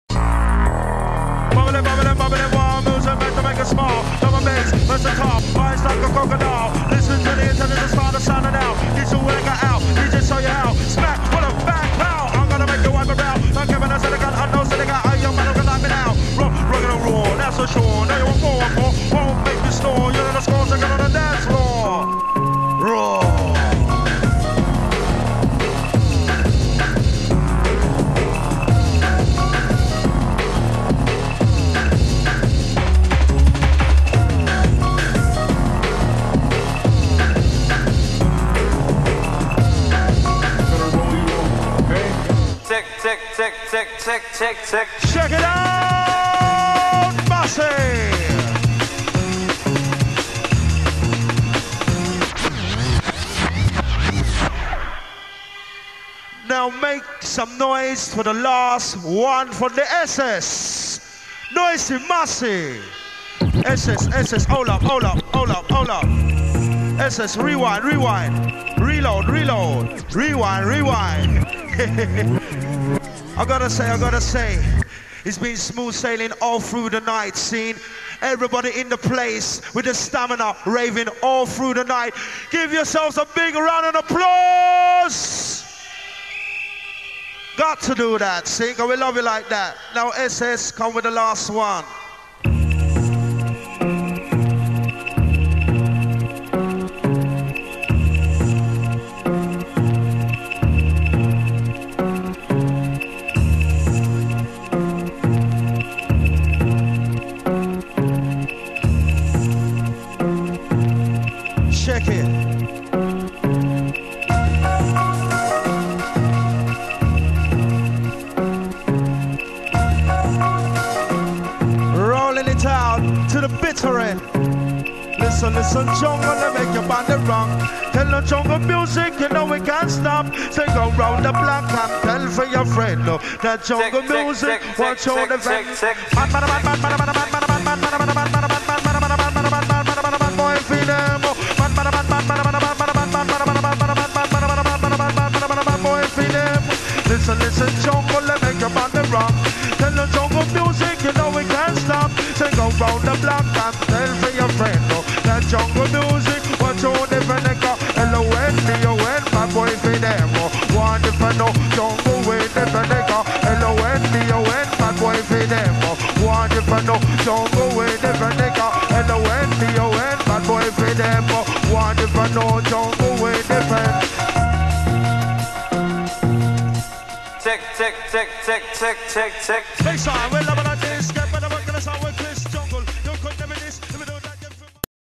Classic STEVIE HYPER D
LIGHTERRRRRRRR

Classic MC Stevie Hyper D alongside DJ SS in 1996.